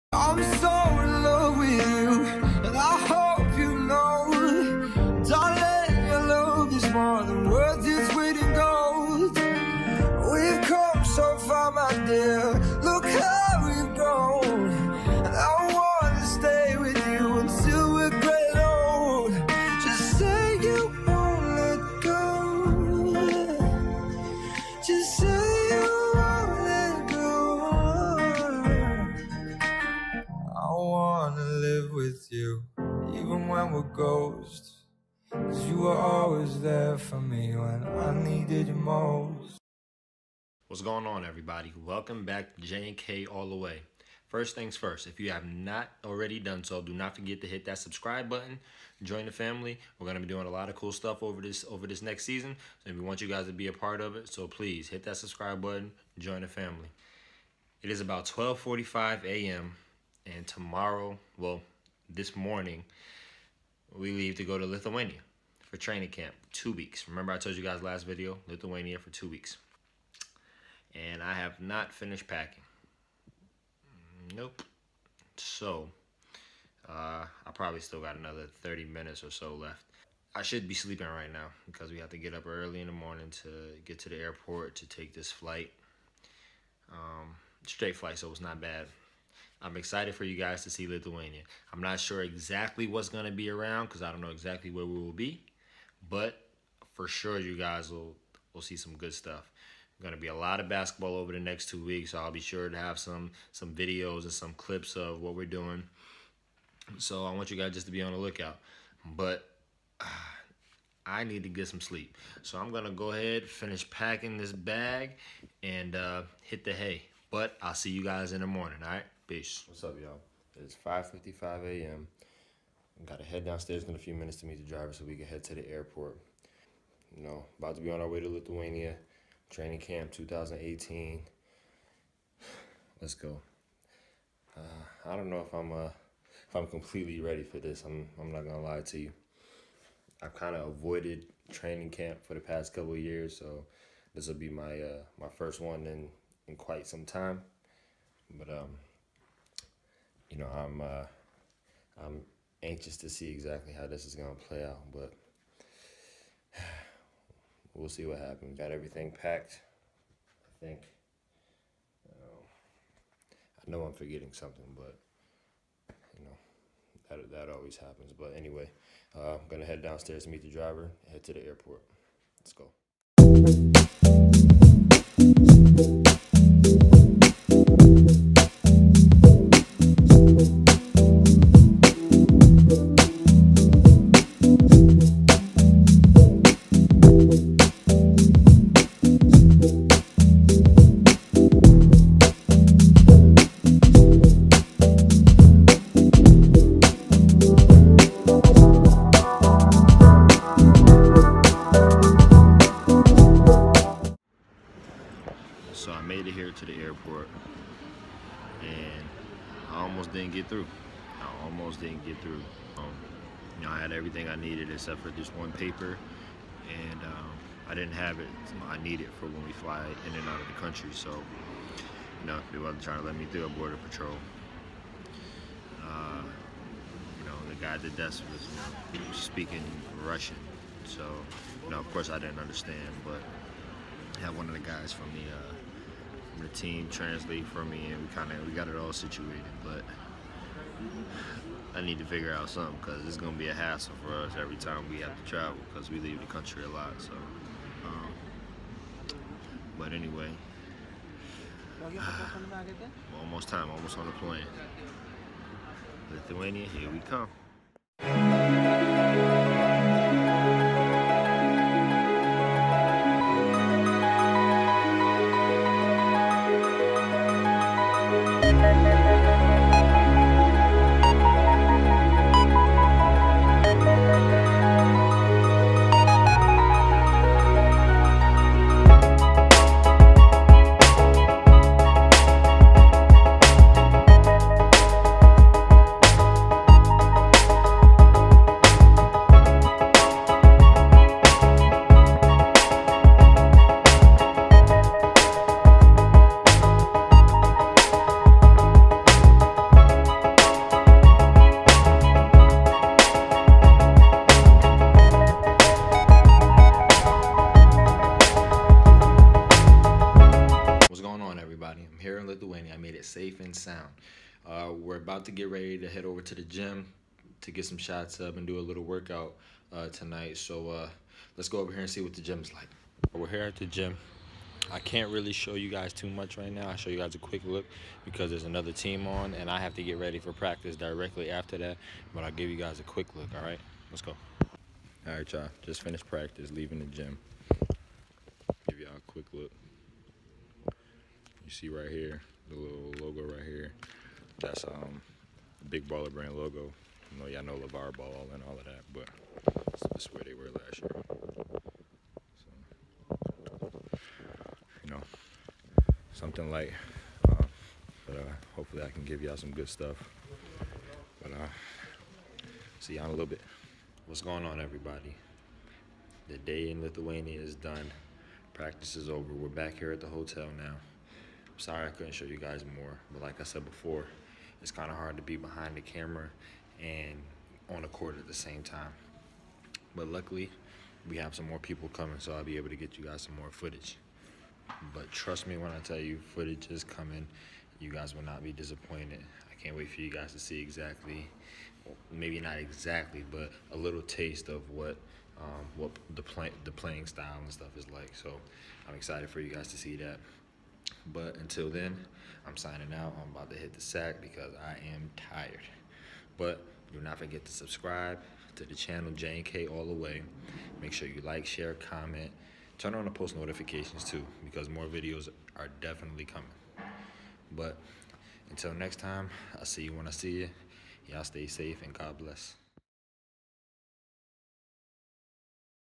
OFF TO LITHUANIA!!
Traveling from Astana to Lithuania for training camp. Its gonna be a long two weeks so i hope you guys are ready!!